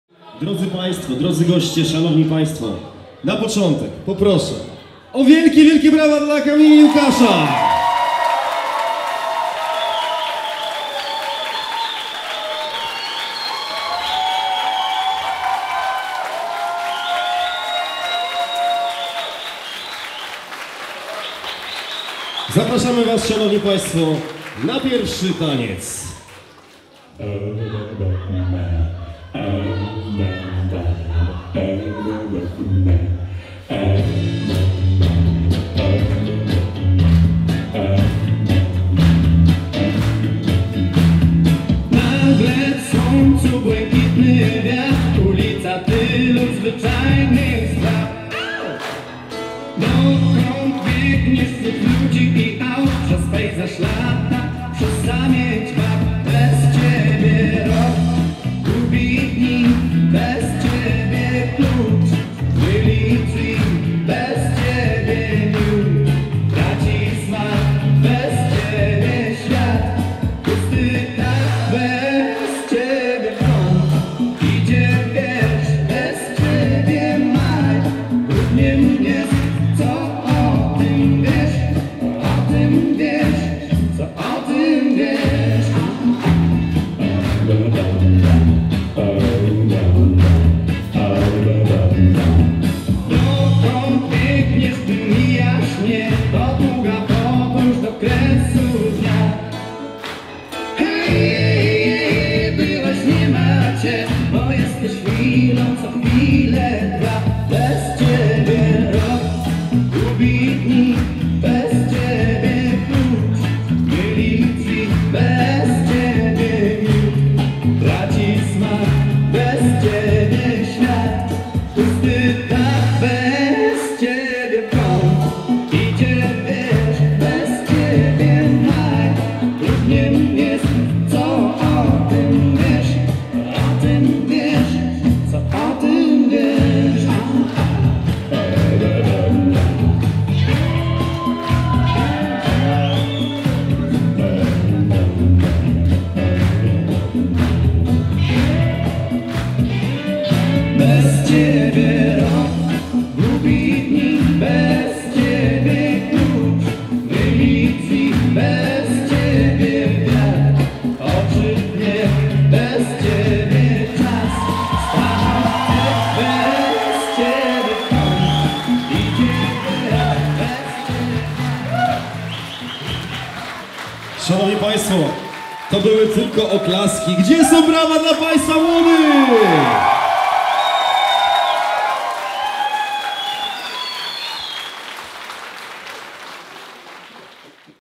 Pierwsze pląsy Kamy i Suchego | Foto-Video Rosiak
Tak zaczęli pląsy na parkiecie Kama i Suchy na swoim wymarzonym weselu